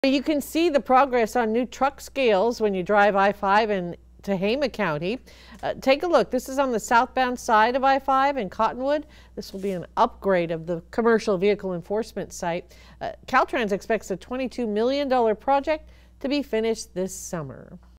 Truck scales progress on I-5 in Tehama County
Caltrans expects the $22,000,000 project to be finished this summer.